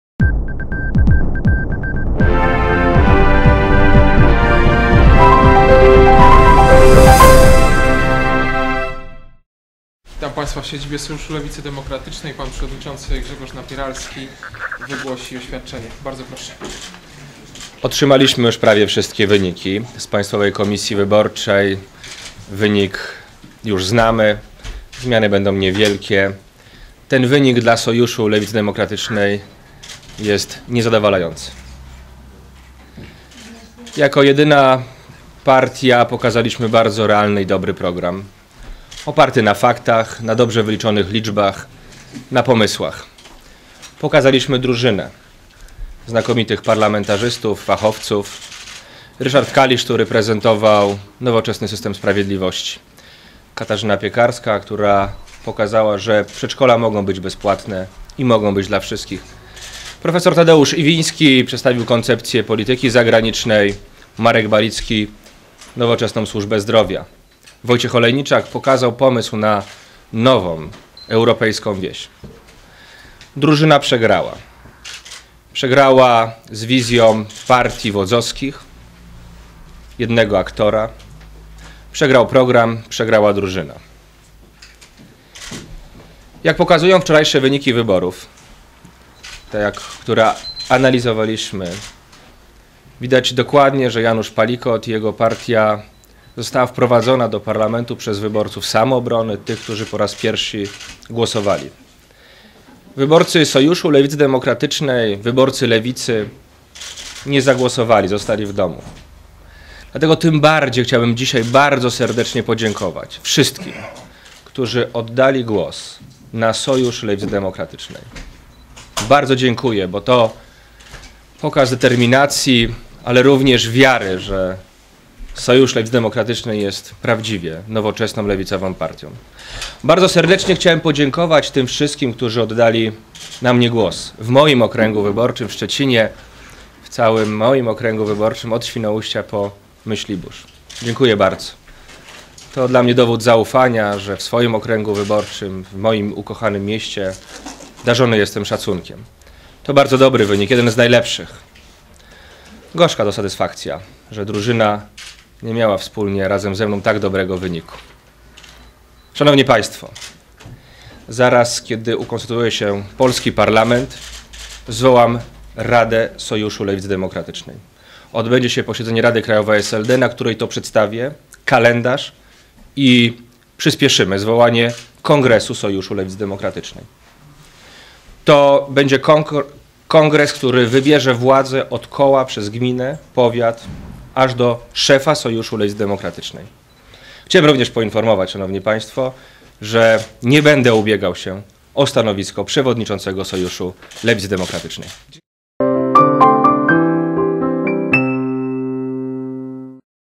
TVSLD : Oświadczenie Grzegorza Napieralskiego
Podczas briefingu prasowe 10 października w siedzibie SLD w Warszawie, przewodniczący Sojuszu, Grzegorz Napieralski podziękował wszystkim wyborcom, którzy oddali głos na SLD i na niego bezpośrednio w Szczecinie.  - Wynik jaki osiągnęliśmy jest  niezadawalający -- powiedział  Napieralski. - Żałuję, że nasz program, dobry program, odpowiadający na wiele dylematów, formułujący rozwiązania na ważne problemy, nie znalazł uznania wśród Polaków.

wiecej